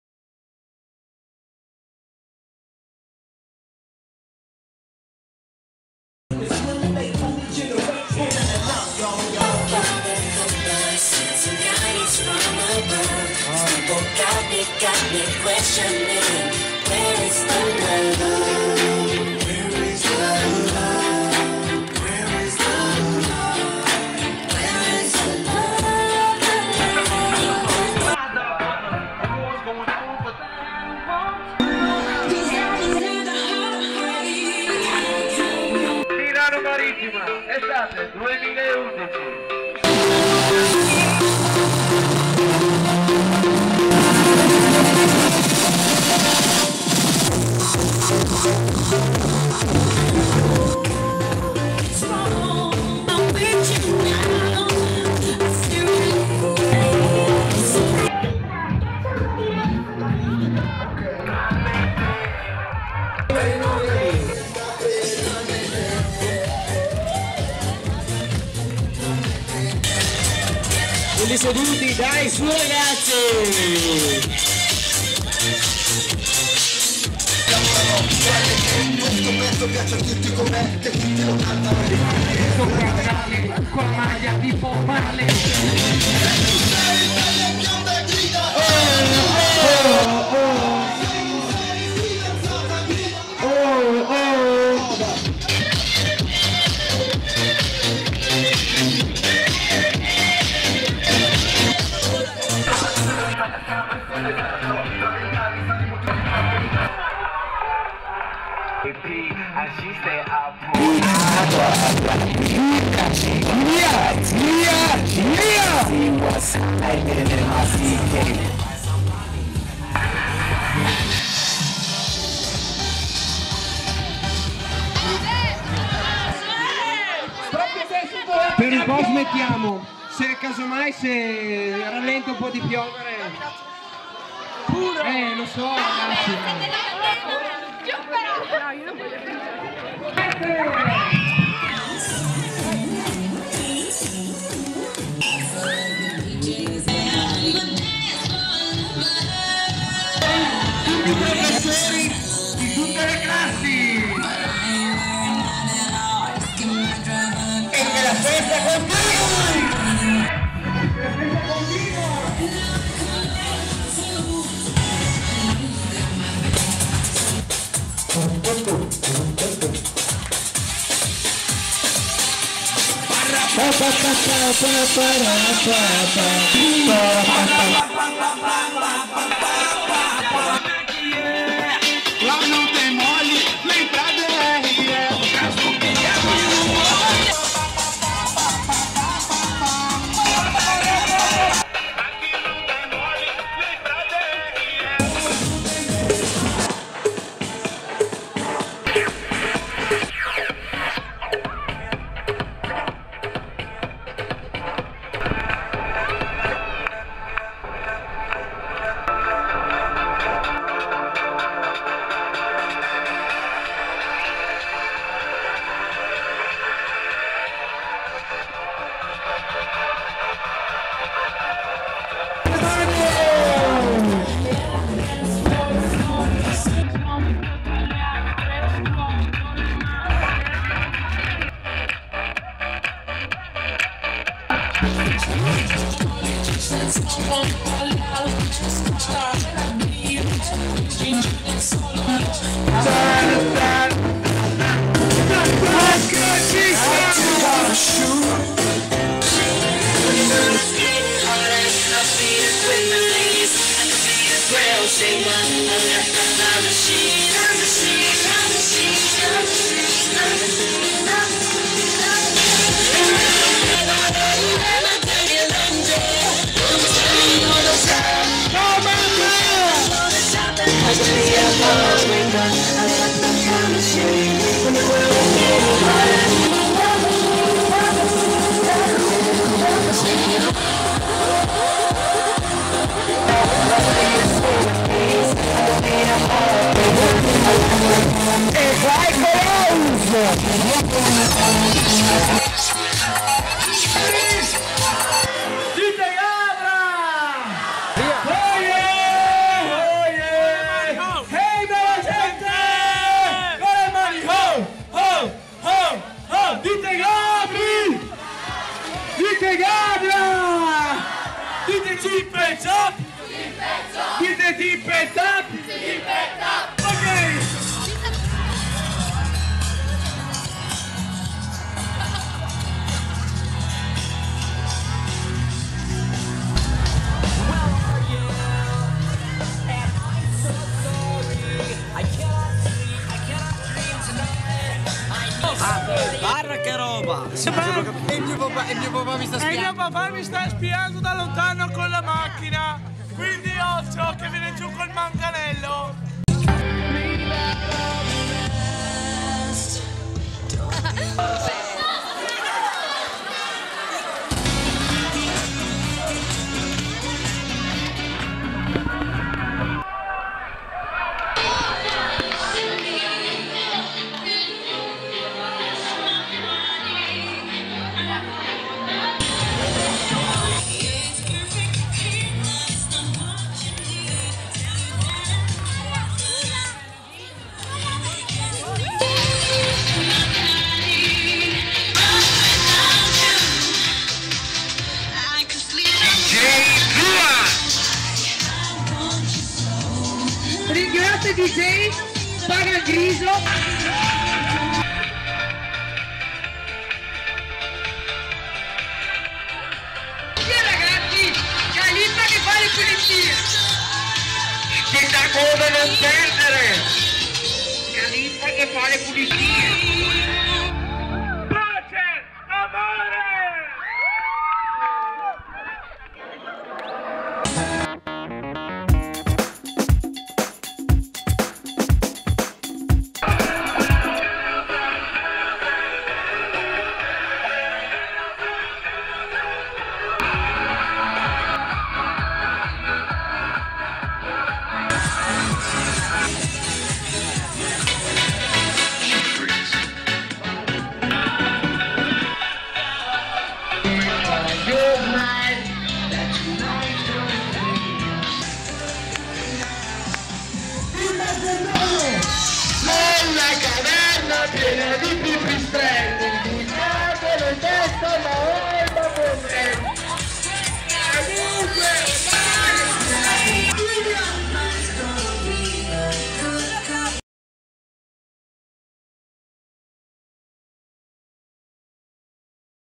ballo_terze_simeoni_2011
Il ballo delle terze nel cortile della scuola media Simeoni di Montorio a conclusione dell'anno scolastico.